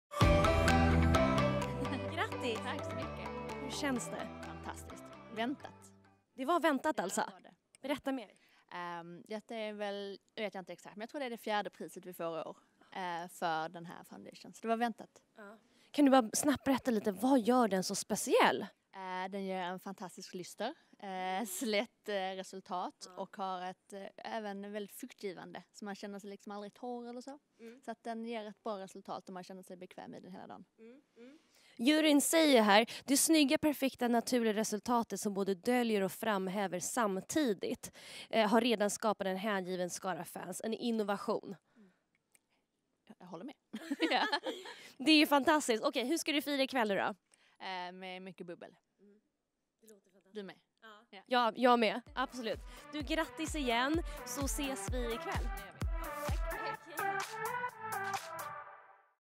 Swedish Beauty Awards 2018: Intervju med vinnaren av årets ansiktsbas över 250 kr!
Här intervjuar vi vinnaren av Årets Ansiktsbas över 250 kr i Swedish Beauty Awards 2018: Sensai Flawless Satin Foundation SPF 20!

Läs mer om Swedish Beauty Awards här